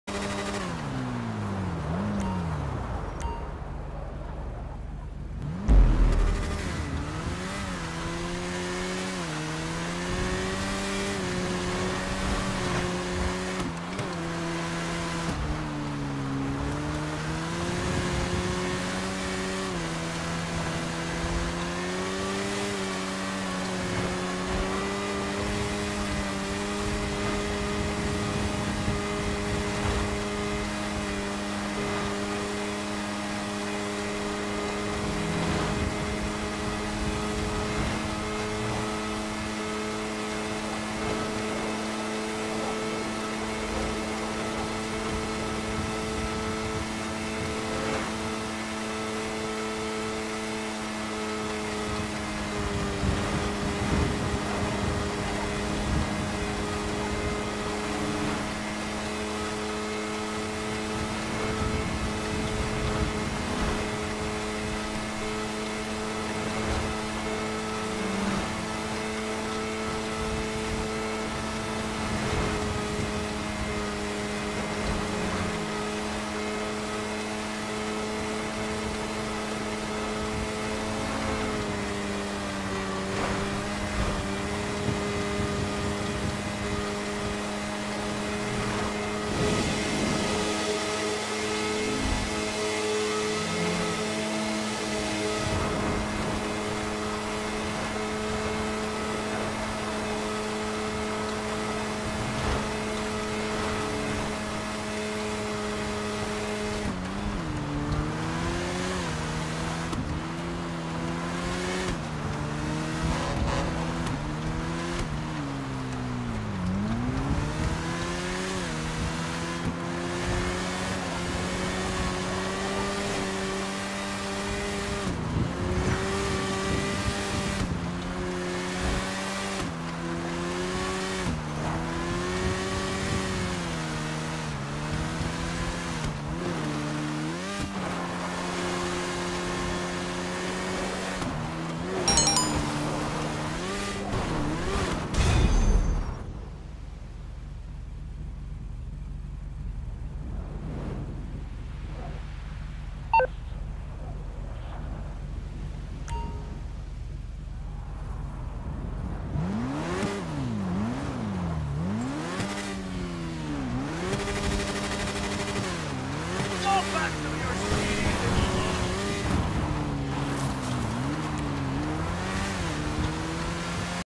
Time Trial: Coast to Coast
Me completing this weeks tim trial first try.  The dragbike never fails me